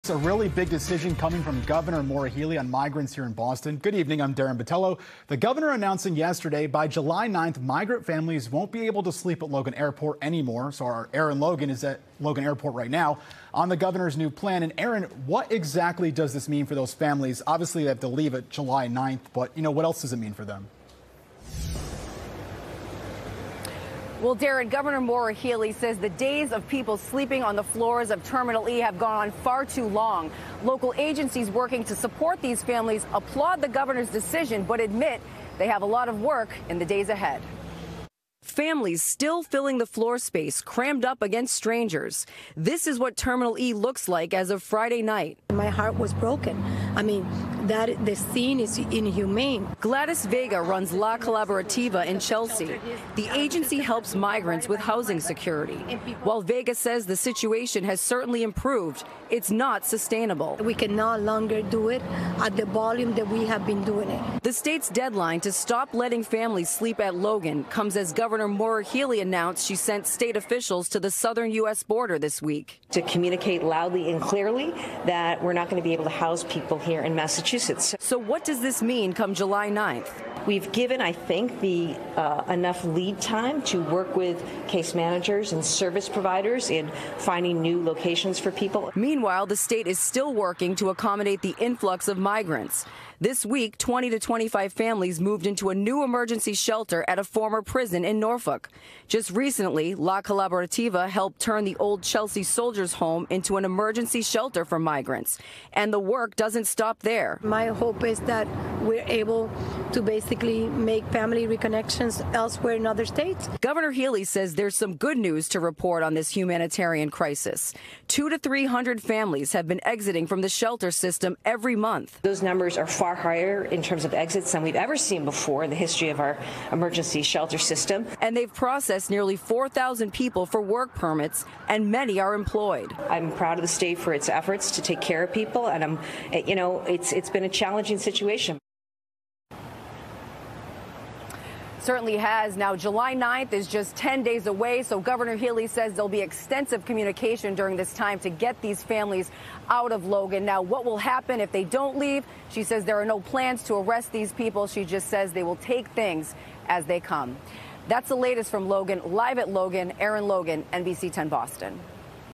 Migrant families banned from sleeping at Logan Airport
Massachusetts Gov. Maura Healey announced Friday that starting July 9, migrants will no longer be allowed to sleep at Logan Airport.